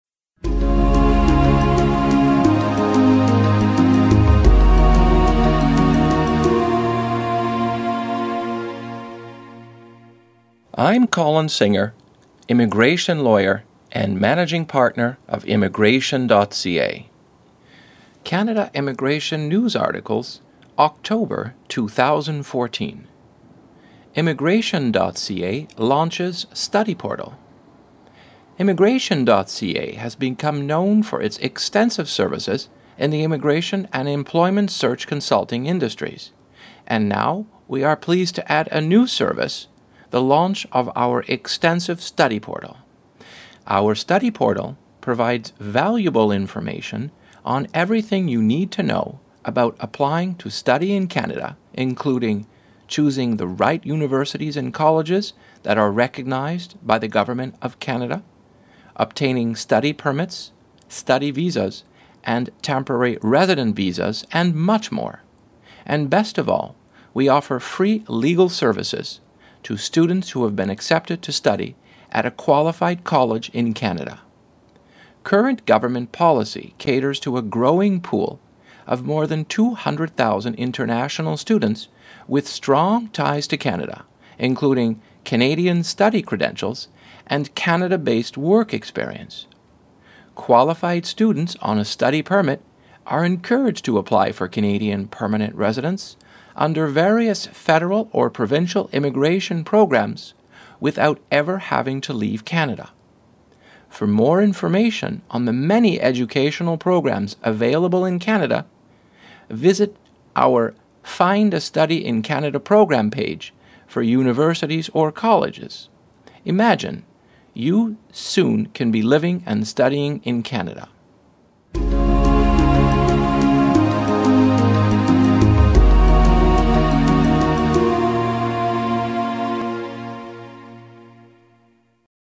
Immigration.ca Launches Study Portal
Immigration.ca has become known for its extensive services in the immigration and employment search consulting industries. And now we are pleased to add a new service – the launch of our extensive Study Portal.